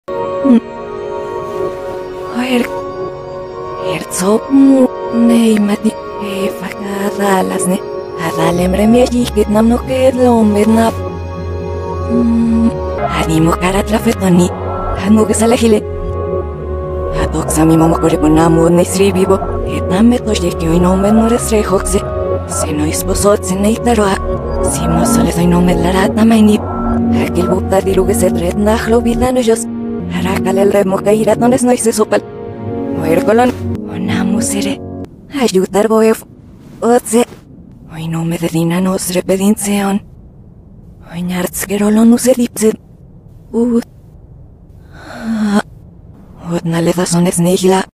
I Reversed Makima's voice in Latin American Spanish
The "Mexikima" is Reversed now! 😂😎

assets are trademarks of the KTS/SVM Networks Group, a division of the Katrin Shevtsov/Satsuki VM Corporation.